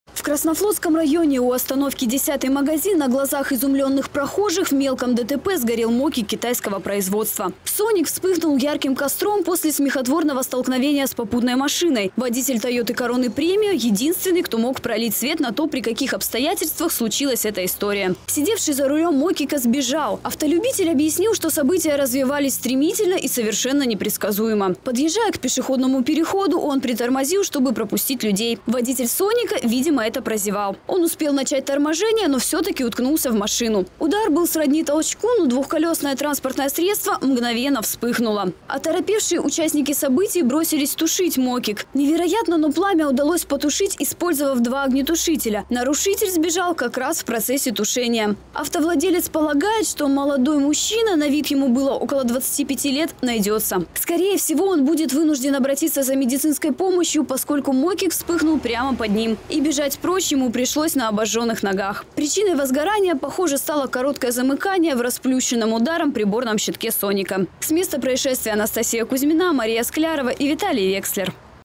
Мопед сгорел после аварии в Хабаровске.MestoproTV
В Краснофлотском районе в мелком ДТП сгорел мокик китайского производства, сообщает портал «Губерния». «Соник» вспыхнул ярким после столкновения с попутной машиной.